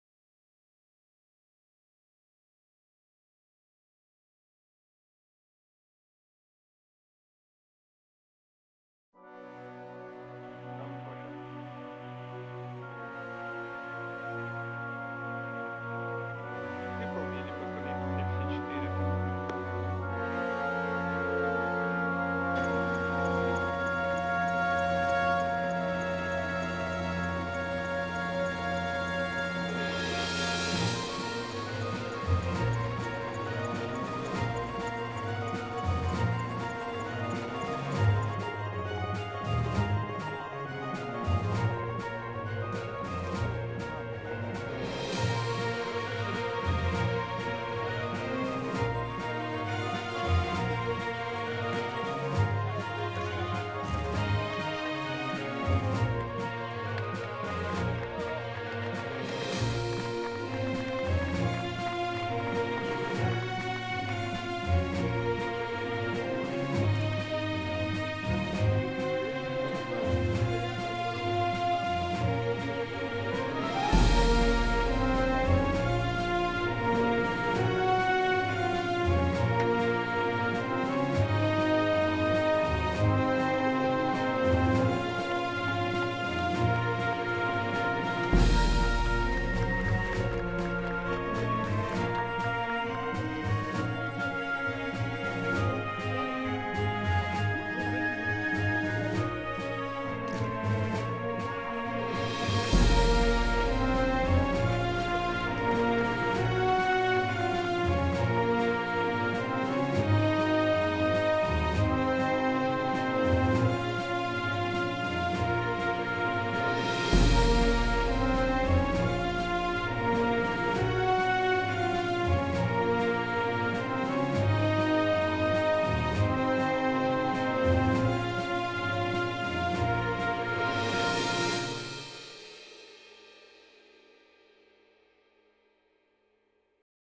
ESA Rientro
Musiche realizzate su un video fornito dall'Ente Spaziale Europeo tramite il col. Roberto Vittori